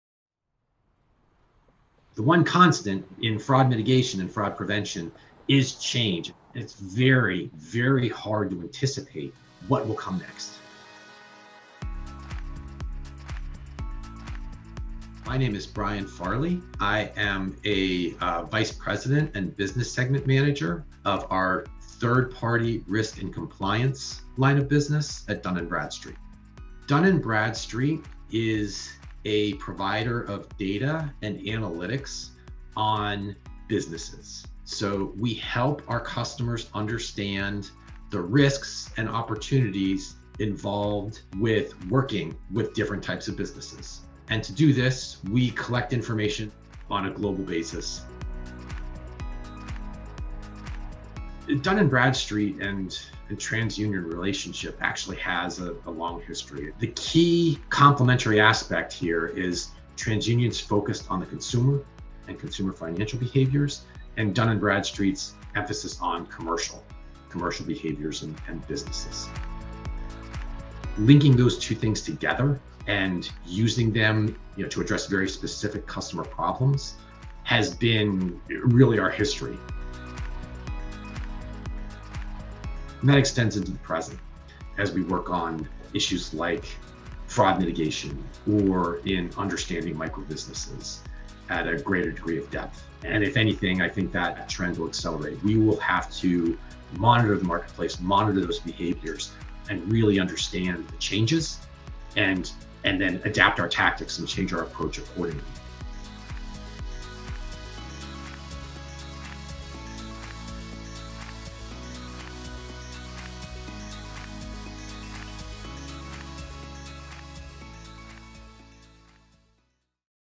Dun & Bradstreet and TransUnion
Understand Risk and Opportunity: learn more about Dun & Bradstreet's long-standing relationship with TransUnion, with key insights on combining data to focus on both consumer and commercial financial behaviors.